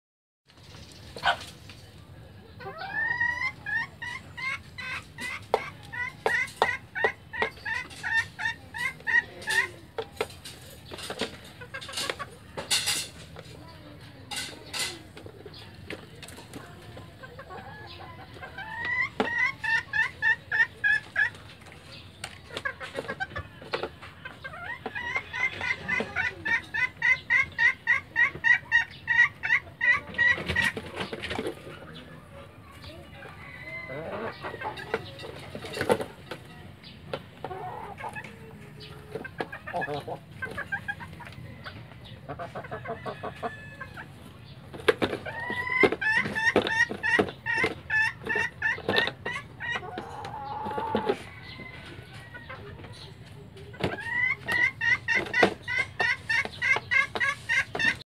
Our tiny best for two pet chicken cage for henny and hehe
Henny and hehe having a great meal of ripe pineapple fruit in their tiny best for 2 pet chicken cage open for our pet bunny peter rabbit.